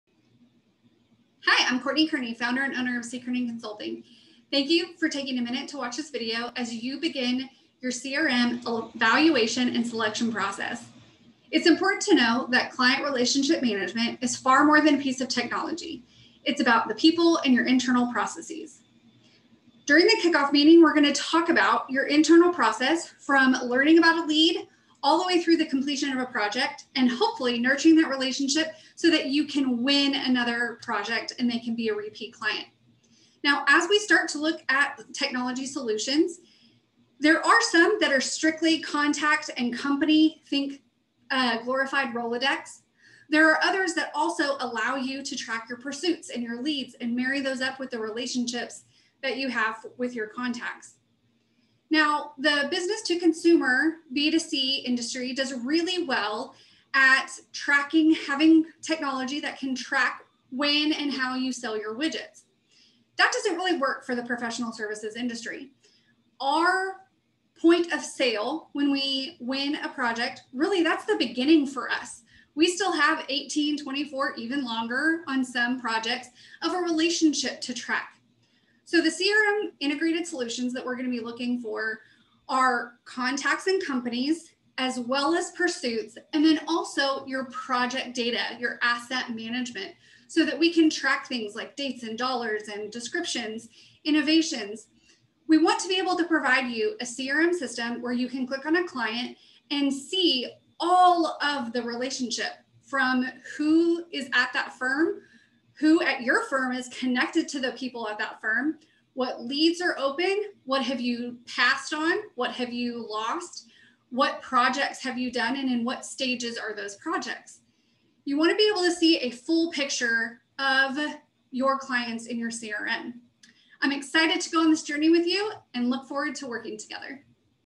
CKC CRM Evaluation and Selection
Founder and Owner, Courtney Kearney, explains the CRM evaluation and selection services CKearney Consulting provides.